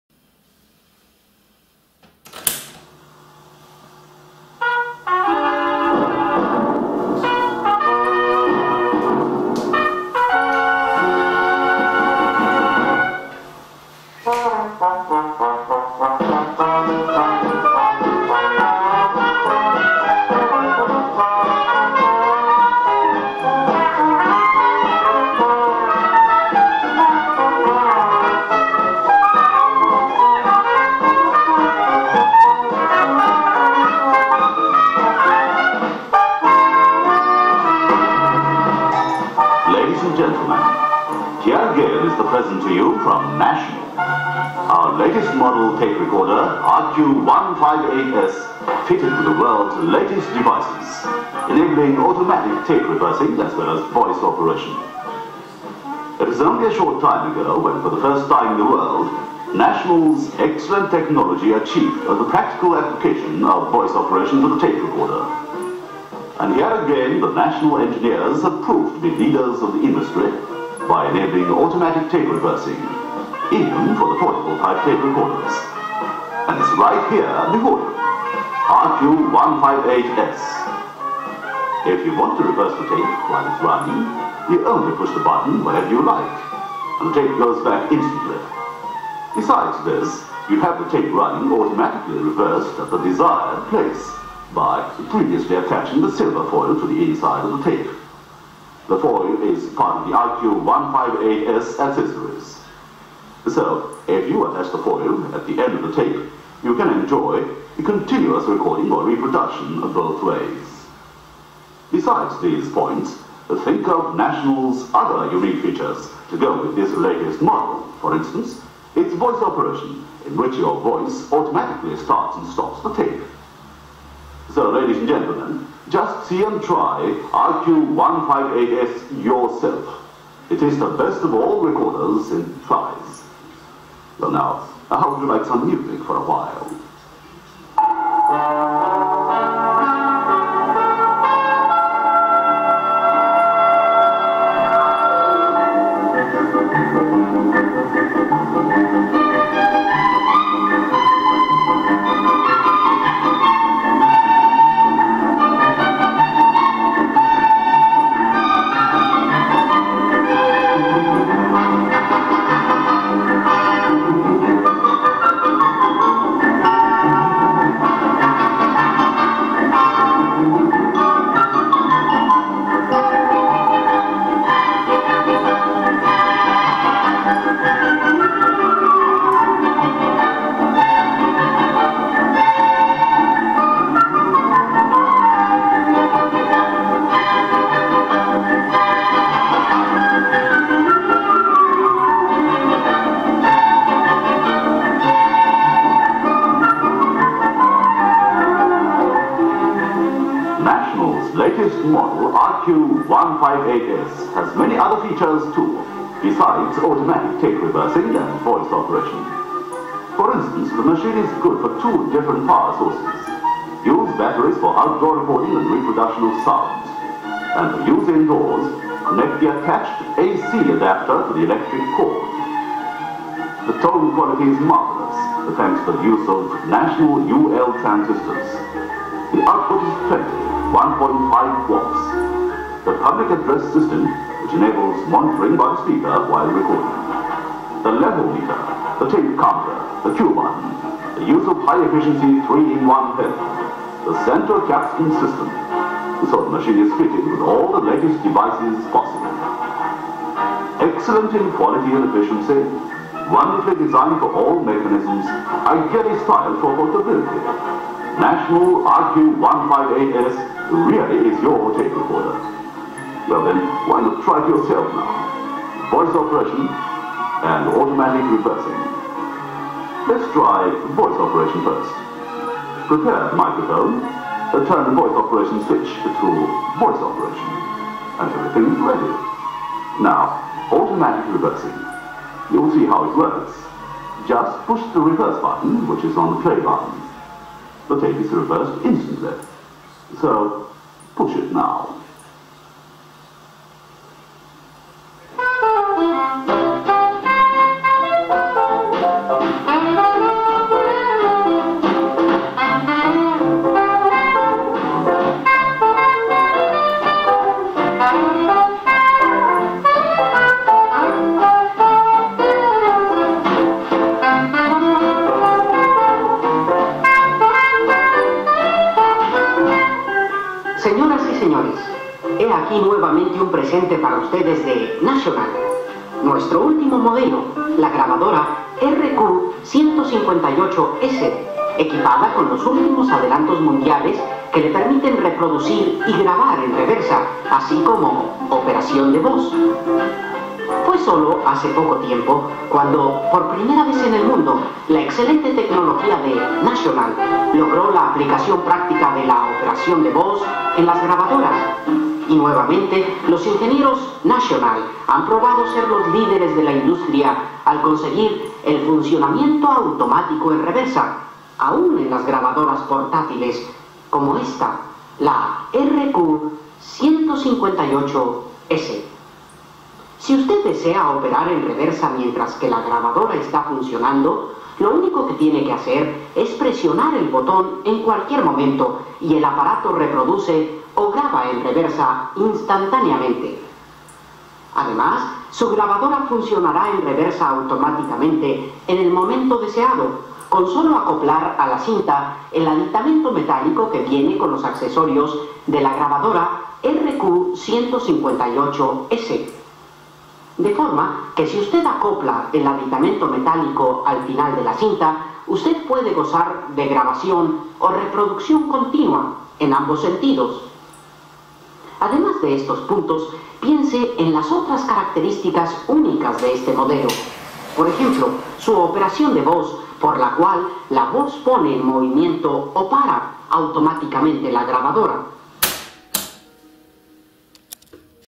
National Panasonic RQ-158S
Demo Video shot on 11/08/14 in advance of my forthcoming Ebay listing for this item.